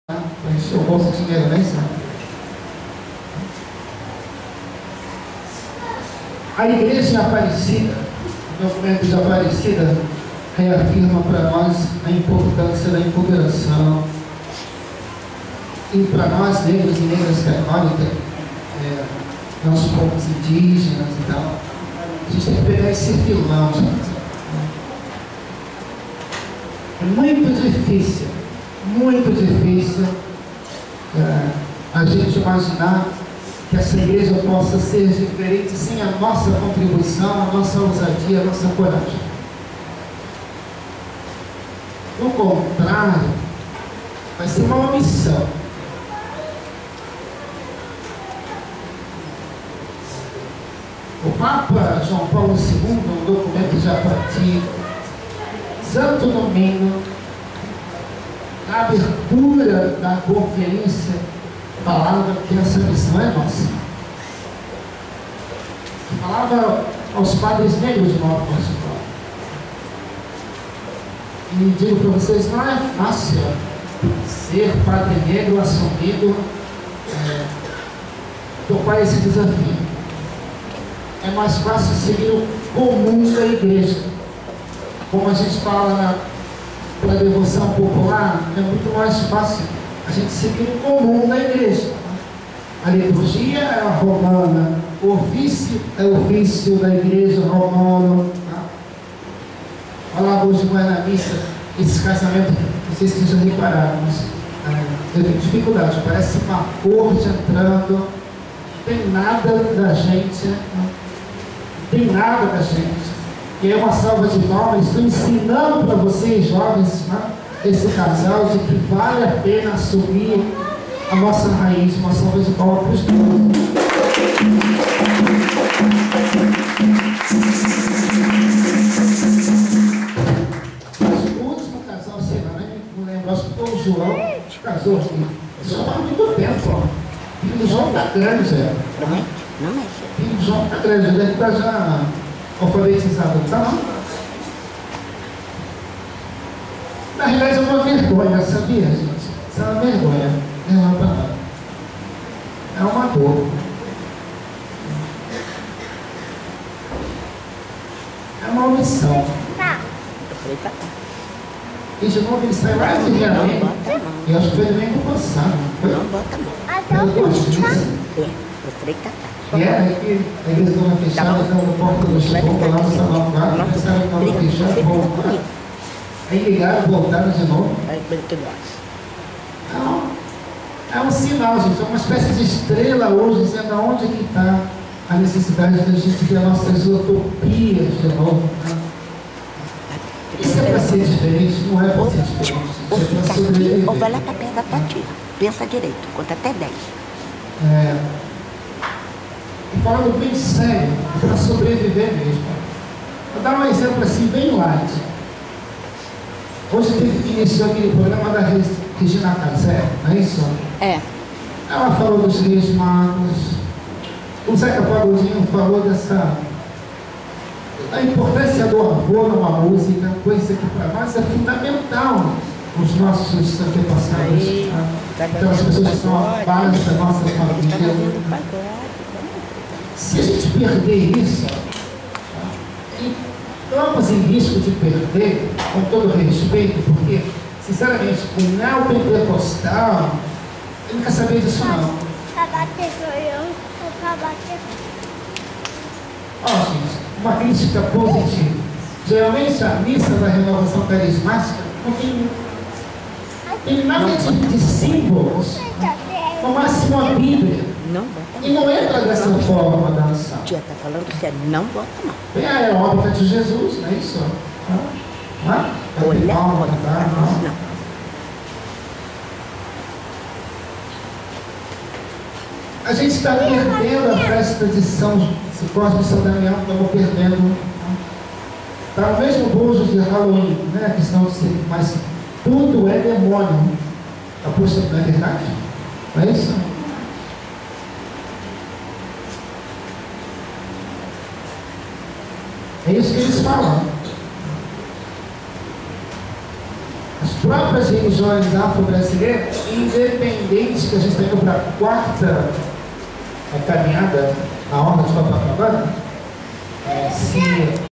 Bodas de prata de Cecilia e Gude Missa Afro Homilia
Missa Afro realizada no dia 2 de janeiro pelo Frei Tatá(Athaylton),OFM, na comunidade São José que faz parte da paróquia matrix de São João de meriti/RJ.
inscreva- se neste canal e veja mais vídeos.
Entre no meu blog e veja mais sobre cultura Afro, Pastoral Afro-Brasileira mais sobre min.